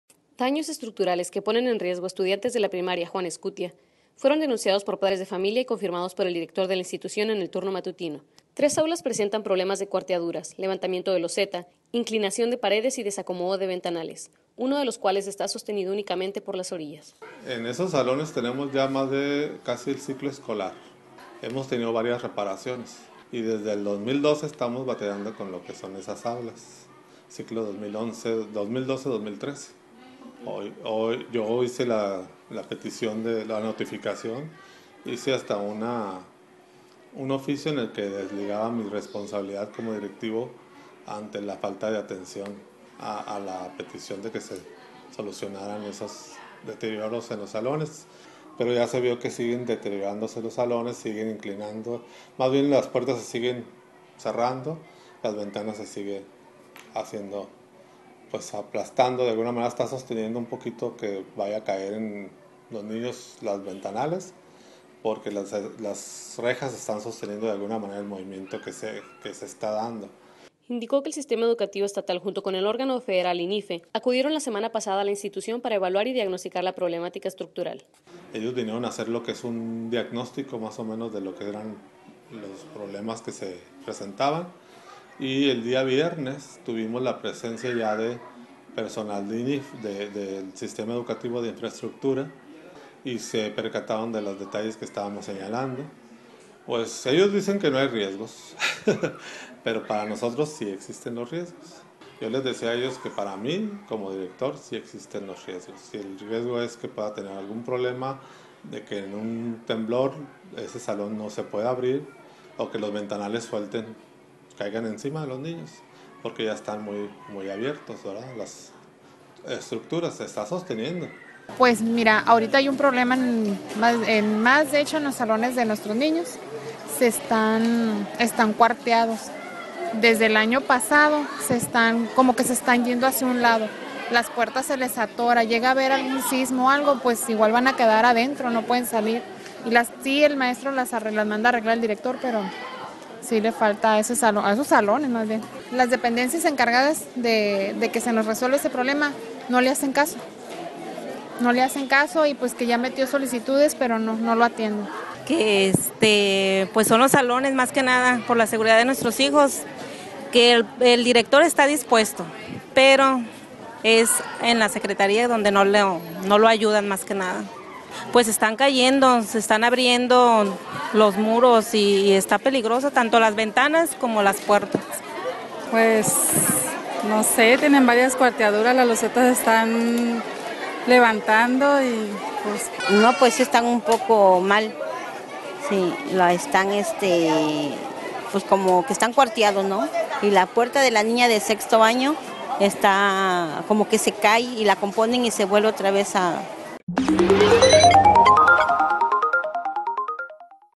Reportan daños estructurales en primaria
Daños estructurales que ponen en riesgo a estudiantes de la escuela Primaria Juan Escutia, fueron denunciados por padres de familia y confirmados por el director de la institución en el turno matutino Miguel Antonio López López.